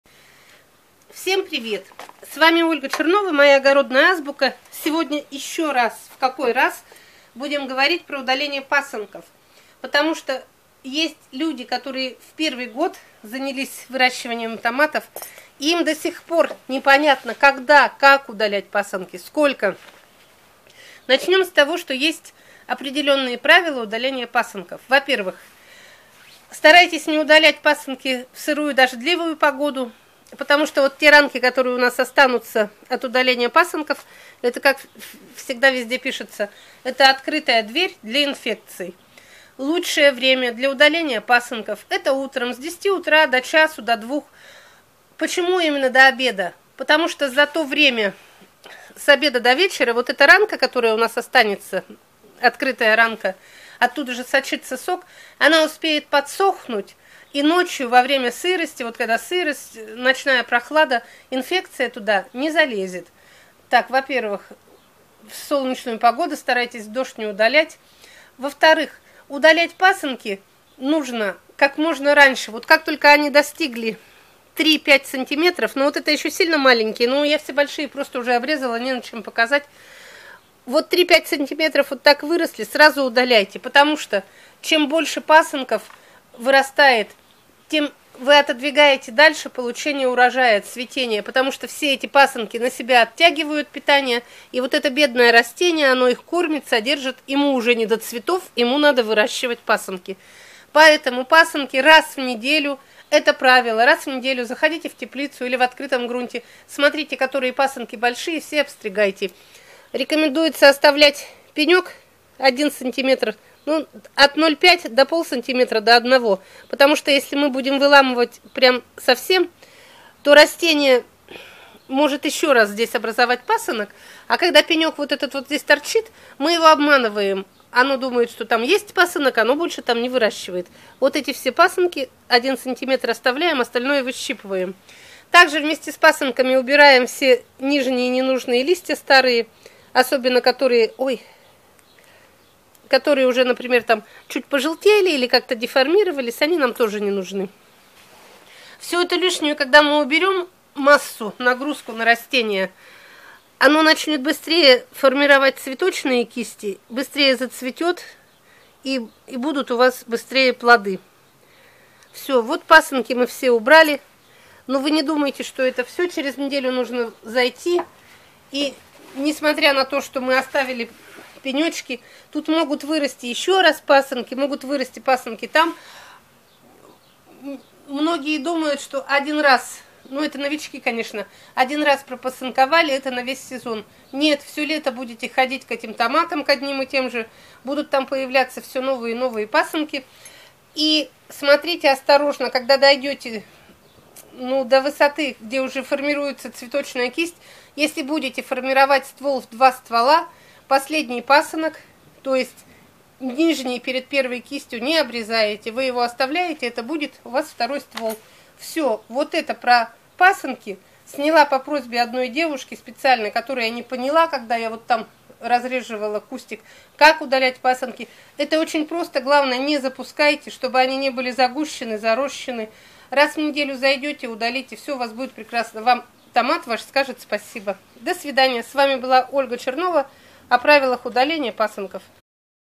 КАК ПРАВИЛЬНО УДАЛЯТЬ ПАСЫНКИ У ТОМАТОВ? Ольга Чернова.
Как правильно удалять пасынки томатов? – это должен знать каждый начинающий огородник.
И хотя опытные огородники делают это все по разному, но все сходятся во мнении, что  пасынкование является одним из самых важных приемов формирования кустов томатов.
Некоторые начинающие думают, что формировать томат – значит вести его в 1 или два ствола.
Но это не так!
Бывалые огородники знают, что формирование – это целый комплекс ухода за томатами, это и пасынкование, и прищипка верхушки, и удаление лишних листьев, и нормирование плодовых кистей…
На этой неделе я решила выложить все весенние и летние  видеоролики на эту тему.
Все их можно найти у меня на канале ЮТУБ, в плейлисте “ФОРМИРОВАНИЕ ТОМАТА”.

КАК ФОРМИРОВАТЬ ТОМАТ В 1 СТВОЛ?
КАК ФОРМИРОВАТЬ ТОМАТ В 2 СТВОЛА?